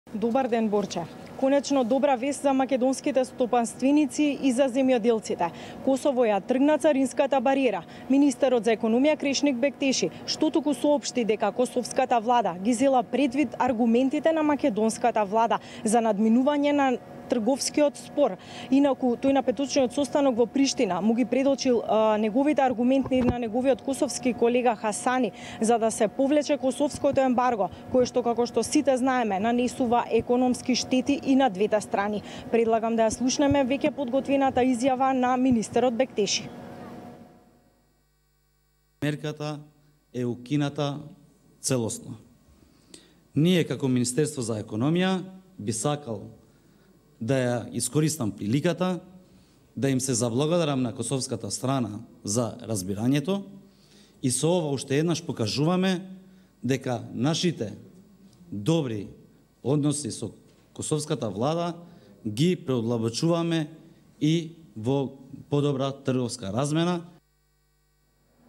Од прес-конференцијата на Бектеши за косовските царини
Косовската влада денеска решаваше за симнување на царините за македонското овошје и зеленчук кои ги воведе на 17-ти јули.